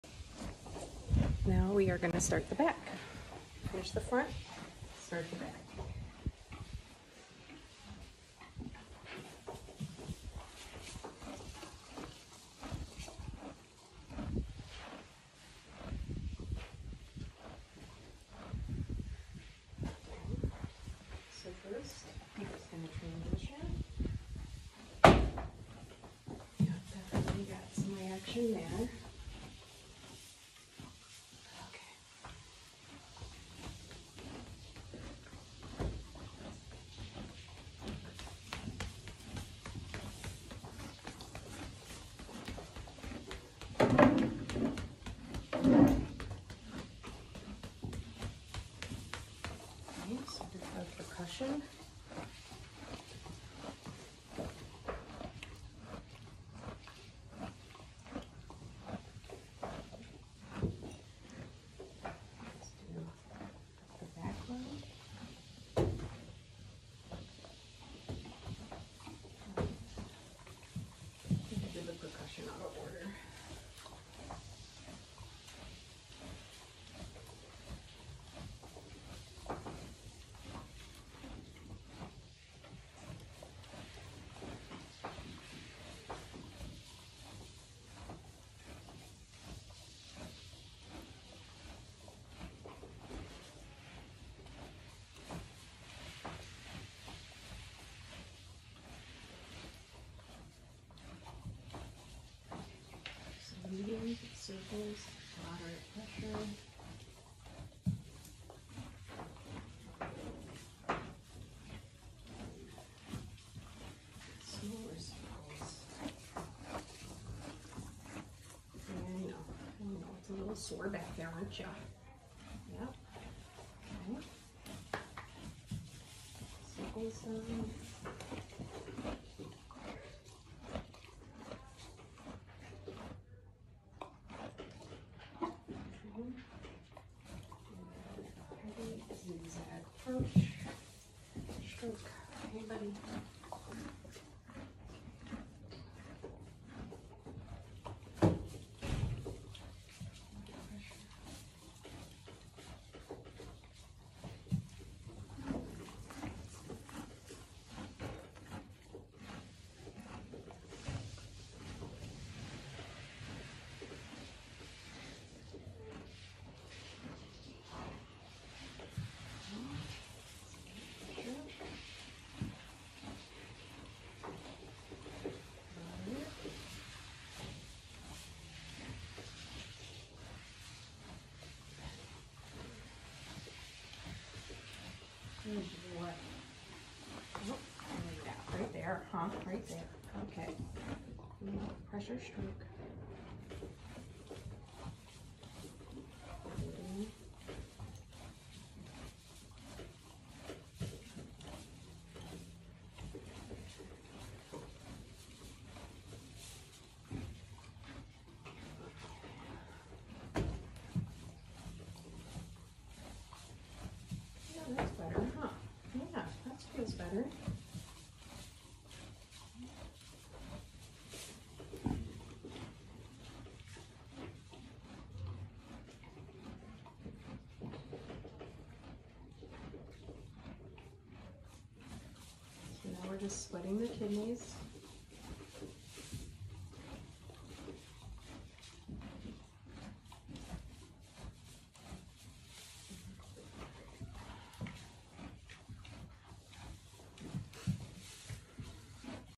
Working Through The Back
In this segment, I show my practice on a horse as I massage through his back and ribs. This horse is particularly reactive telling me he will need more than one massage to work through his soreness and tension.